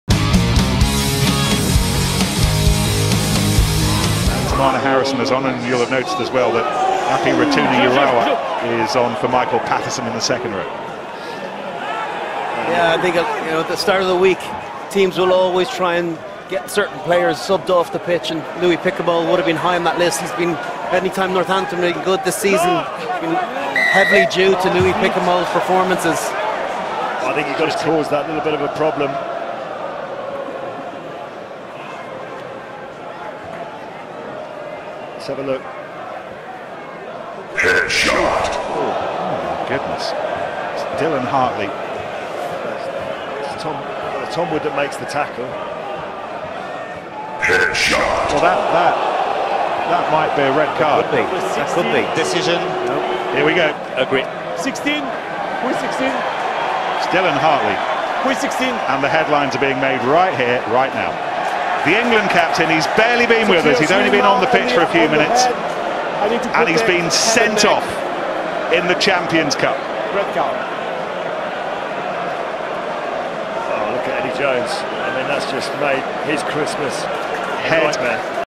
Dylan Hartley met une grosse droite à Sean O'Brien (Carton rouge)
Le capitaine du XV de la Rose s'est pris un carton rouge logique après avoir mis une grande piche à Sean O'Brien.

Si vous voulez me payer une bière en guise de remerciement